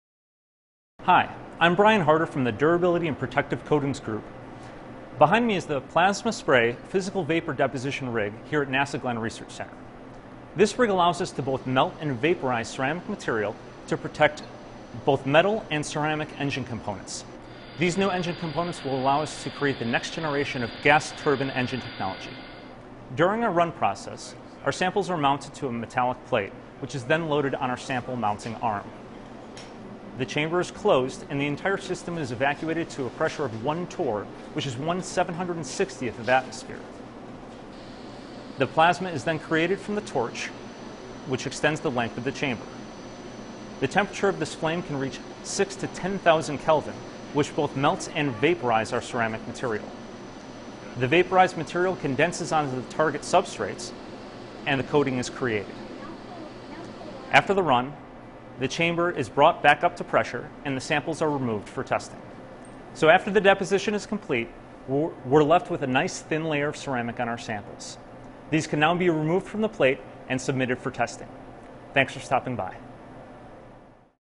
Super Thin Ceramic Coatings
'New technology being developed at NASA's Glenn Research Center creates super thin ceramic coatings on engine components. The Plasma Spray -- Physical Vapor Deposition (PS-PVD) rig uses a powerful plasma flame to vaporize ceramic powder, which condenses onto the component and forms a uniform ceramic coating. Bryan Harder, the lead for the PS-PVD, demonstrates the innovative technology.'